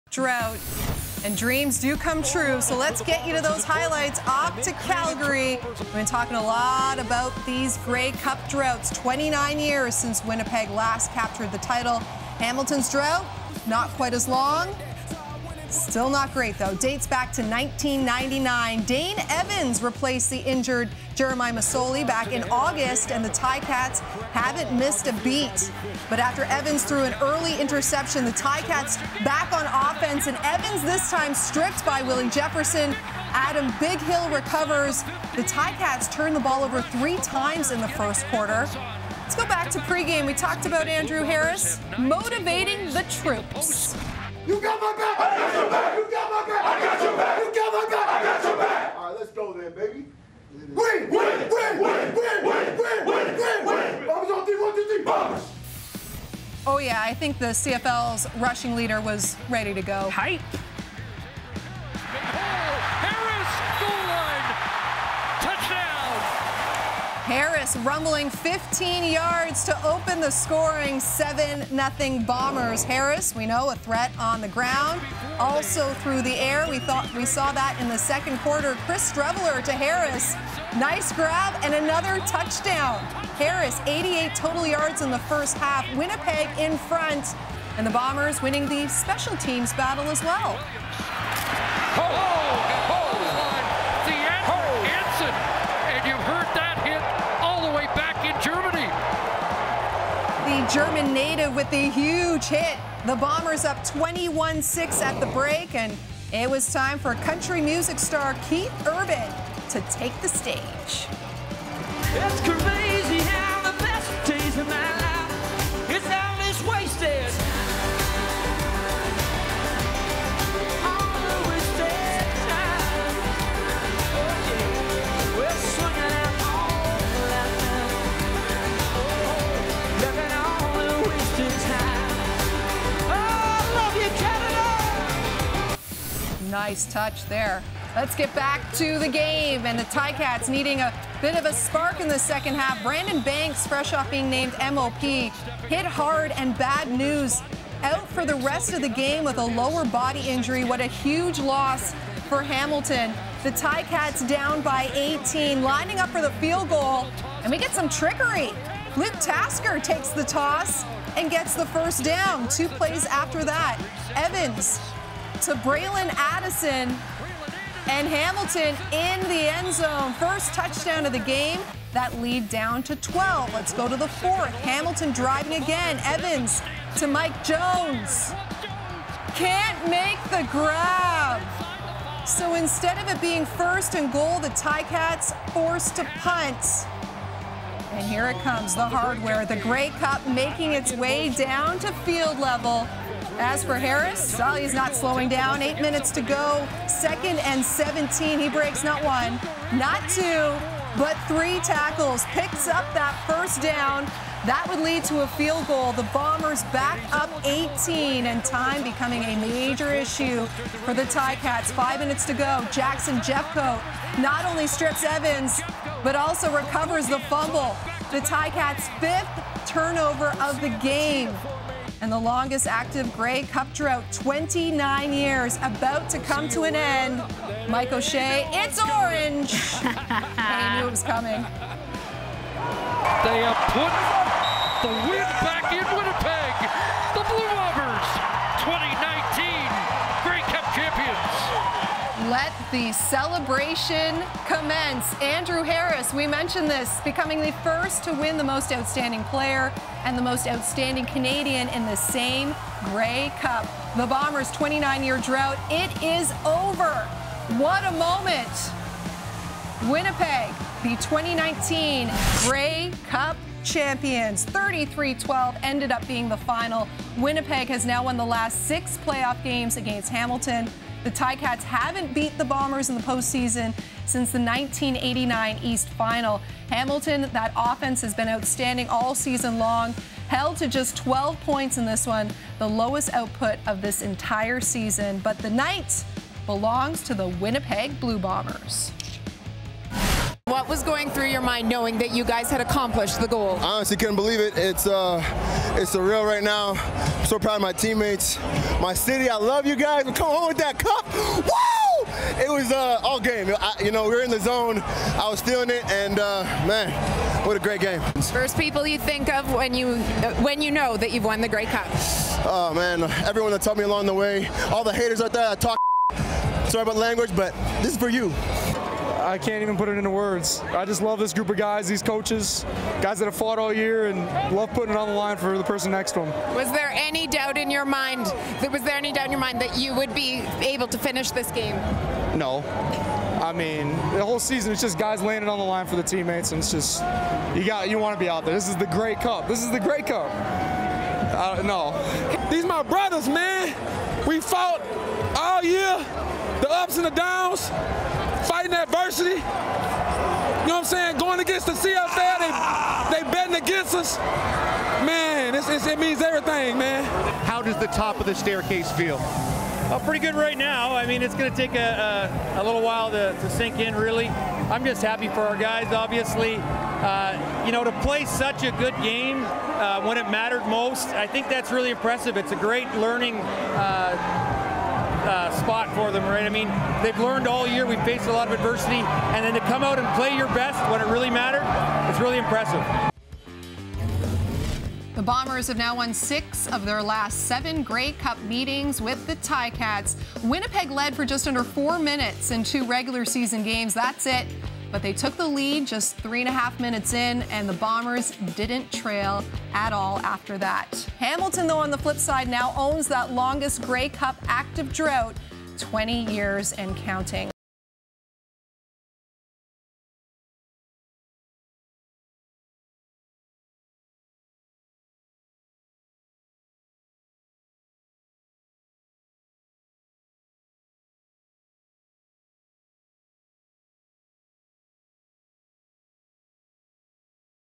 Bombers snap 29 year drought, bring home their first Grey Cup since 1990
Andrew Harris carried the load for Winnipeg with 134 rushing yards and 35 receiving yards with two touchdowns to lead the Bombers to their first Grey Cup win in 29 years. Harris became the first Canadian in CFL history to be named the Most Valuable Player and Most Valuable Canadian of the Grey Cup game.